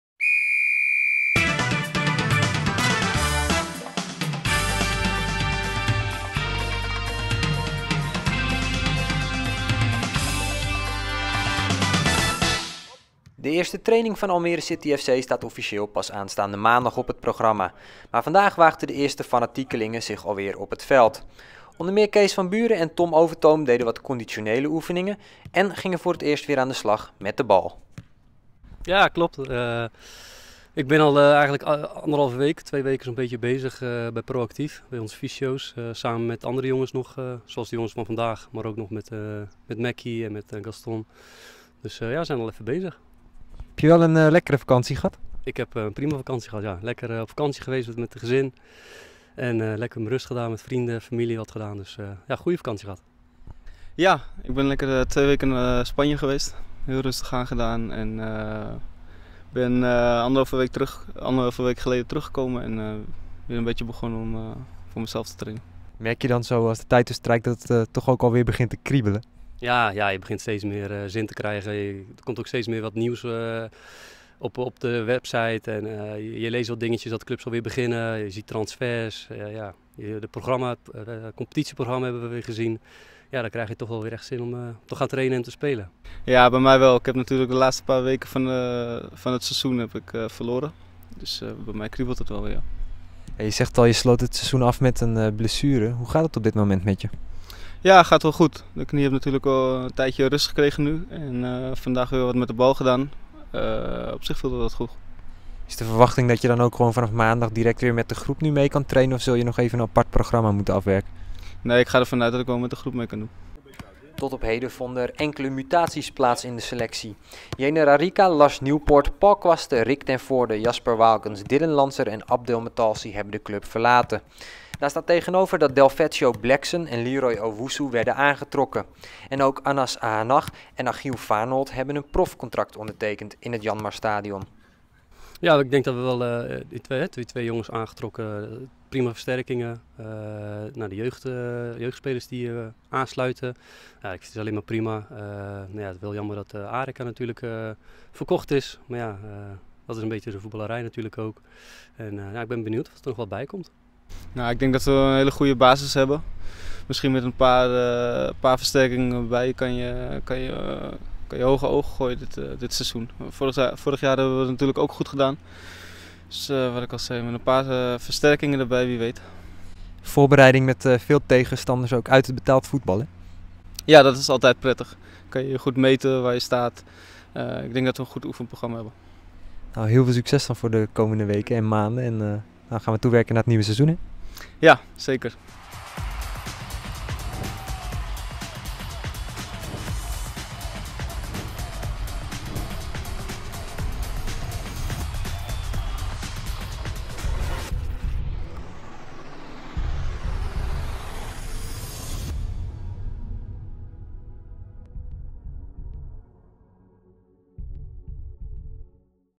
Almere City TV | Fanatiekelingen alweer op het veld
De eerste training van Almere City FC staat pas aanstaande maandag op het programma, maar vandaag waagden de eerste spelers zich alweer op het veld. Onder meer Kees van Buuren en aanvoerder Tom Overtoom deden enkele conditionele oefeningen en gingen met de bal aan de slag. 'Natuurlijk kriebelt het weer.'